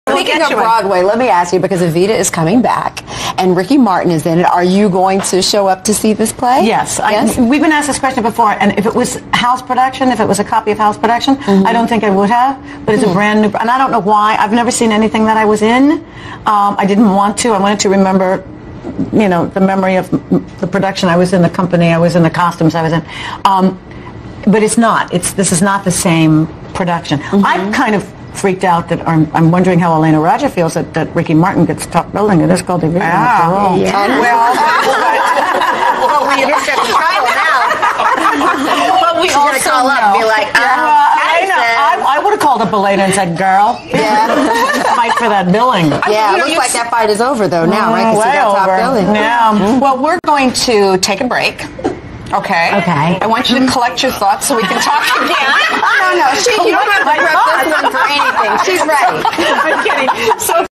Patti LuPone Interview Clip Evita Elena Roger Ricky Martin
An interview clip in which Patti talks about the Elena Roger/Ricky Martin Evita revival.